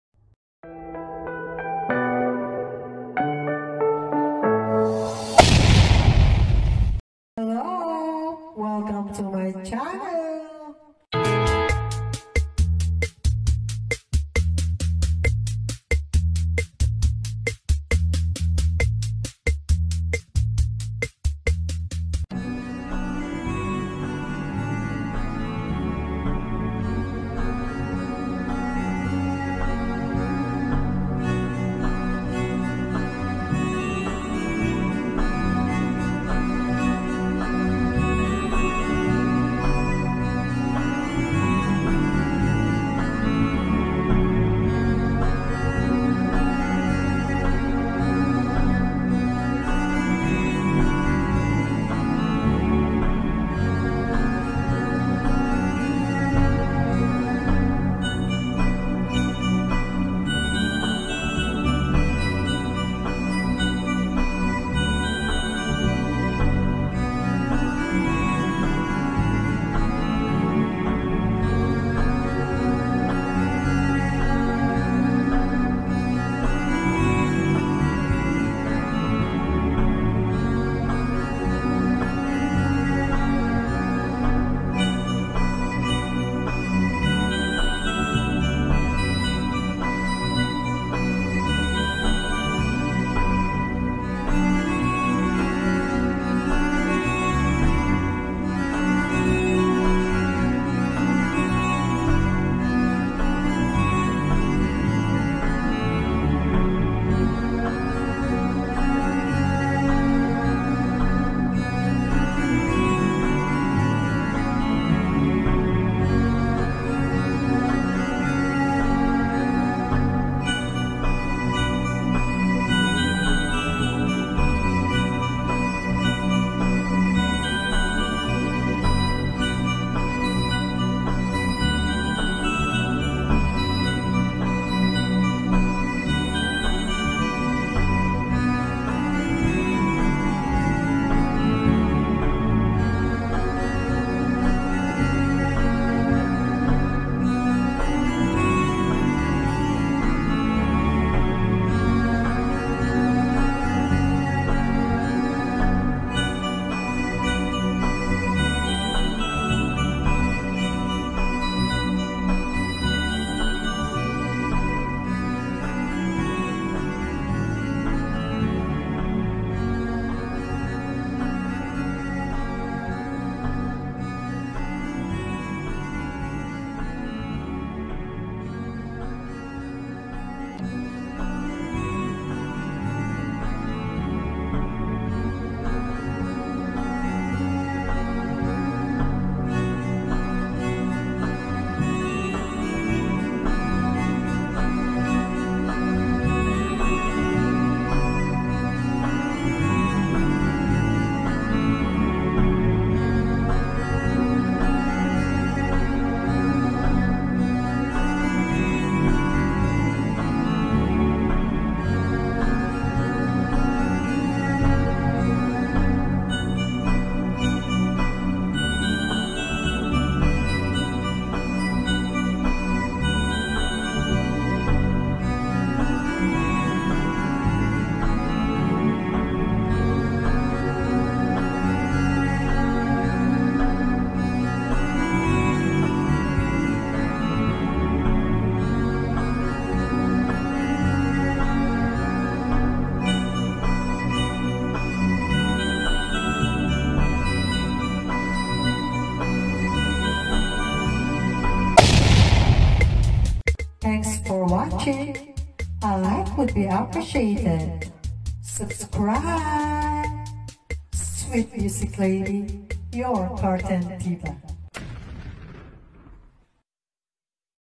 DENIKKA'S BEDROOM ZEBRA HELLO KITTY ROOM made by:SWEET MUSIC LADY.CURTAIN DIVA
MY DAUGHTER'S SIMPLE ROOM TOUR FOR THE MONTH OF OCTOBER 2021.. ZEBRA DESIGN. I WISH THIS ROOM WAS BIG FOR DECORATION AND MANY THINGS TO PUT I HOPE YOU LIKE IT GUYS THANKS TO ALL MY LEGIT FRIENDS FOR ALWAYS BACK ON MY CHANNEL.. I WILL RETURN ALL OF YOUR SUPPORT TO ME AND ALSO MY KIDS CHANNEL @DENEVVA MUSICA​ ,,I LOVE YOU ALL..
LYKA... SWEETMUSICLADY

MUSIC BACKGROUND by: @Andrea Michele Vincenti​  ANDREA MICHELE VINCENTI